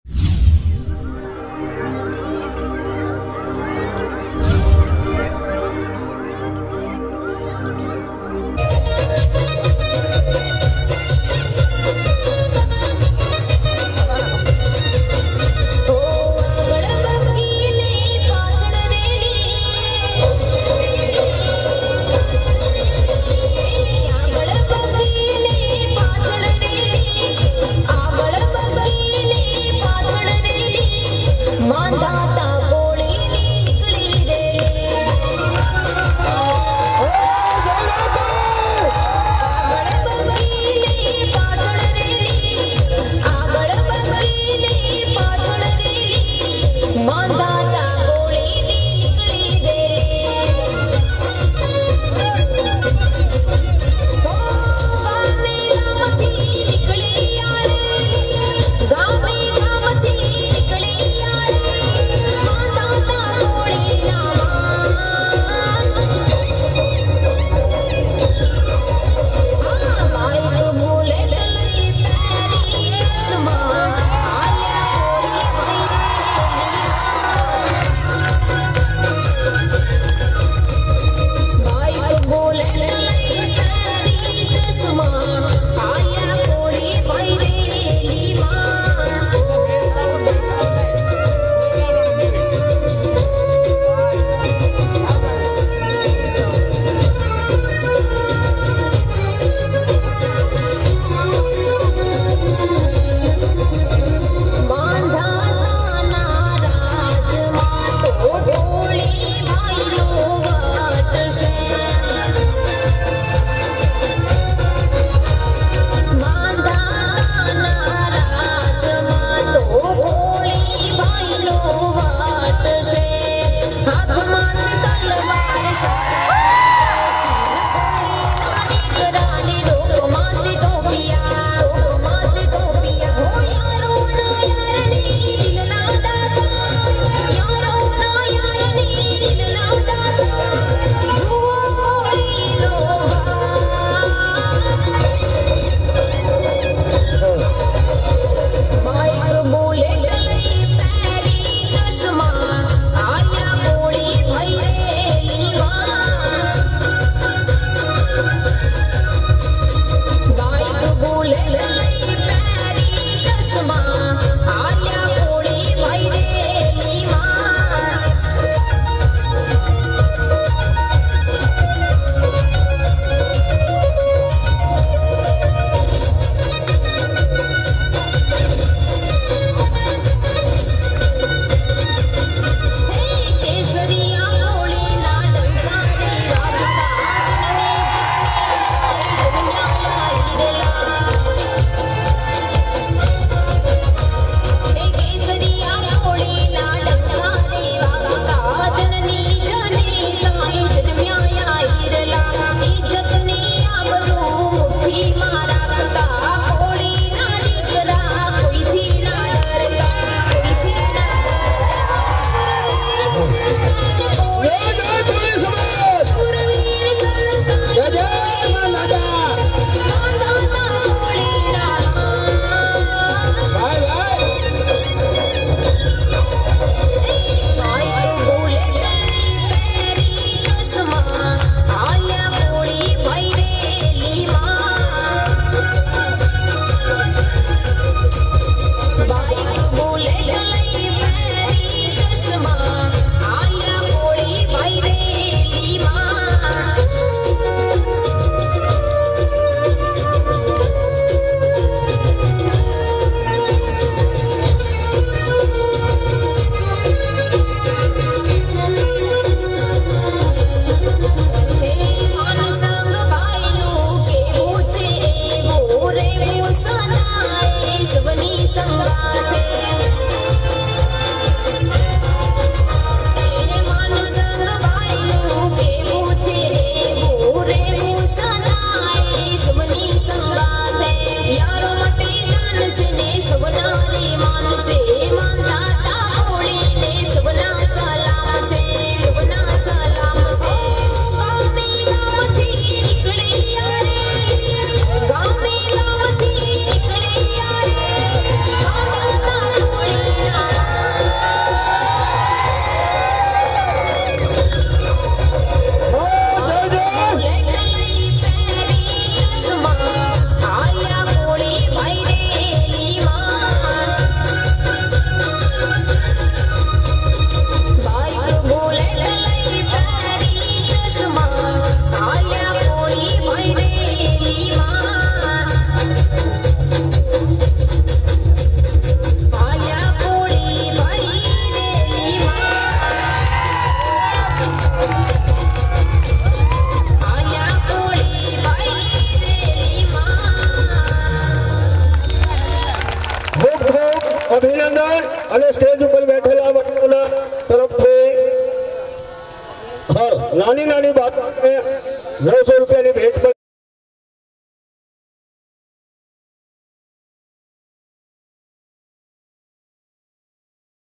Mandhata Koli Ni Nikli Reli II Vitthalpara To Bhadena II Jay Mandhata II 2020
jay mandhata

jay koli samaj

vitthalpara to Bhadena

maha reli 2019

mamta studio